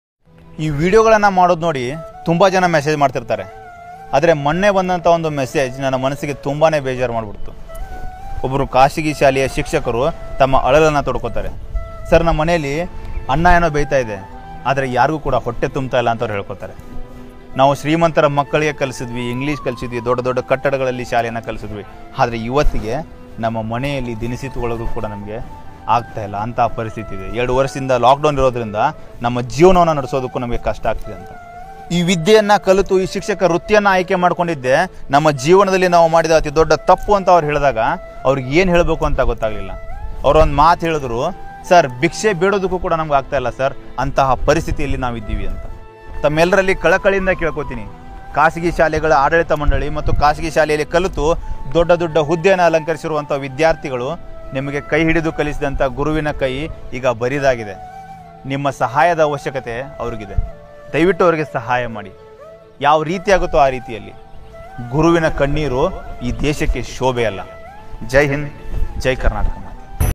No Salary, No food, Private Schools & College, Teacher's Lecturers What a bad situation to teachers?
bad situation to teachers